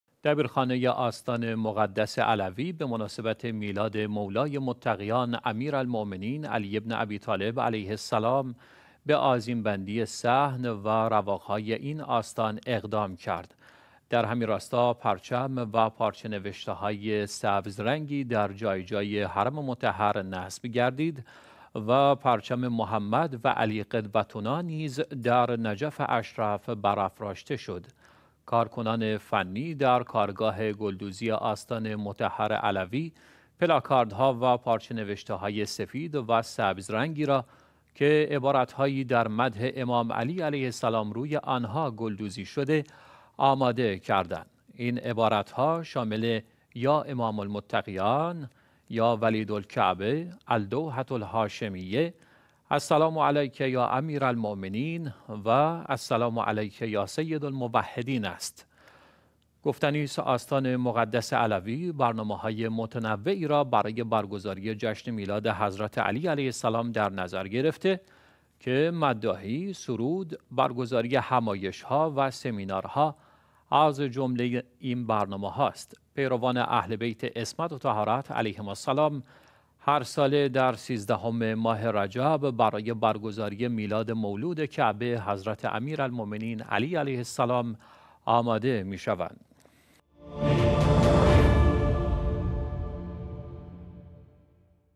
آماده سازی حرم مطهر امیرالمومنین علی علیه السلام برای ایام ولادت
دبیرخانه آستان مقدس علوی به مناسبت میلاد مولای متقیان، امیرالمومنین، علی بن ابی طالب علیه السلام، به آذین بندی صحن و رواق های این آستان اقدام کرد.
در همین راستا پرچم و پارچه نوشته‌های سبز رنگی در جای جای حرم مطهر نصب گردید و پرچم «محمد و علی قدوتنا» نیز در نجف اشرف برافراشته شد.
کارکنان فنی در کارگاه گلدوزی آستان مطهر علوی، پلاکاردها و پارچه نوشته های سفید و سبز رنگی را که عبارت هایی در مدح امام علی علیه السلام روی آنها گلدوزی شده، آماده کردند.
این عبارت‌ها شامل یا امام المتقیان، یا ولید الکعبه، الدوحة الهاشمیه، السلام علیک یا امیرالمومنین و السلام علیک یا سیدالموحدین است.
گفتنی است، آستان مقدس علوی، برنامه های متنوعی را برای برگزاری جشن میلاد حضرت علی علیه السلام در نظر گرفته که مداحی، سرود، برگزاری همایش ها و سمینارها از جمله این برنامه هاست.
پیروان اهل بیت عصمت و طهارت علیهم السلام هر ساله در ۱۳ ماه رجب، برای برگزاری میلاد مولد کعبه، حضرت  امیر المومنین علی علیه السلام آماده می شوند.